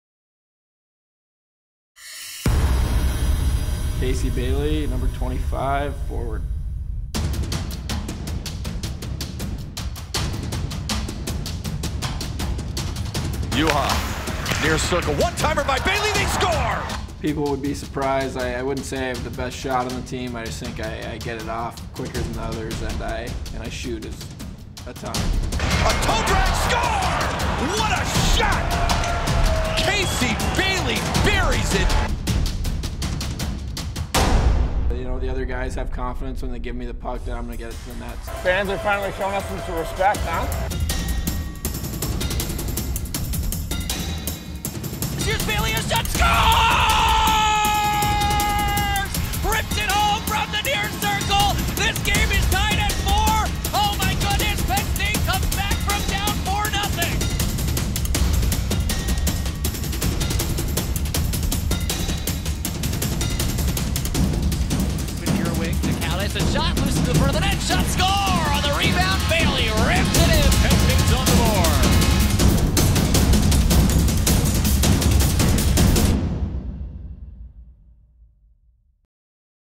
Penn State Hockey Hobey Baker Spotlight - Casey Bailey
Penn State Men's Hockey Jr. Forward Casey Bailey & Sr. Forward Taylor Holstrom have been nominated for the 2015 Hobey Baker Award, the Heisman of Hockey.

Vote daily for Casey & Taylor at hobeybaker.com